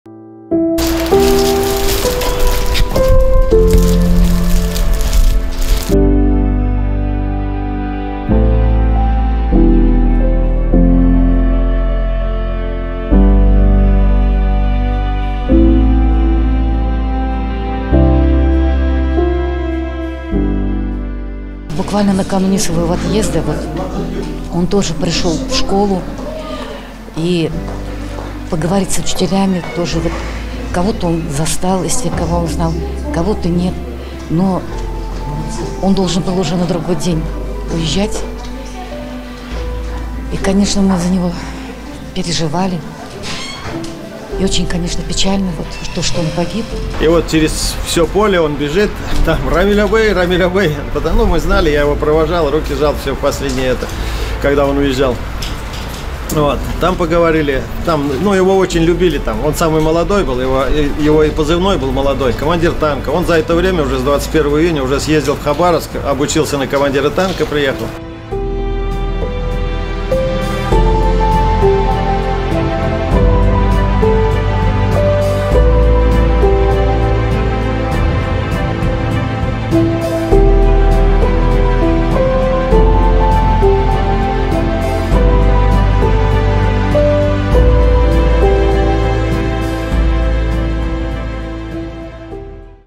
Имя командира танка Алексея Денисова увековечили в Нижней Мактаме
В Альметьевском районе открыли памятную доску бойцу СВО Алексею Денисову. Он был командиром танка. Погиб в городе Рубежное Луганской народной республики при выполнении воинского долга.